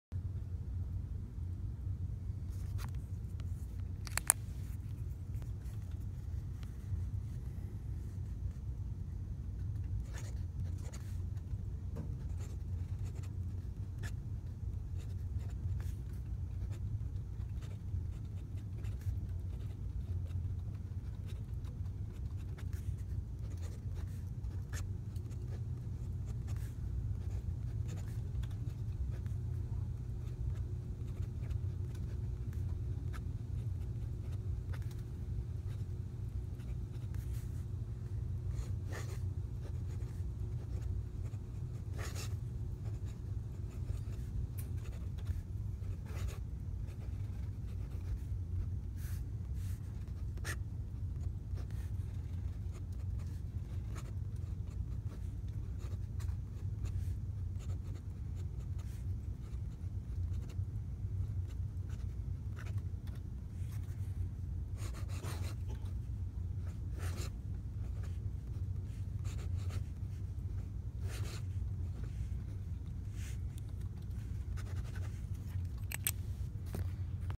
Pilot Vanishing Point Matte Black with Architect Grind
Nib started out as a 18k Fine and was ground by Mark Bacas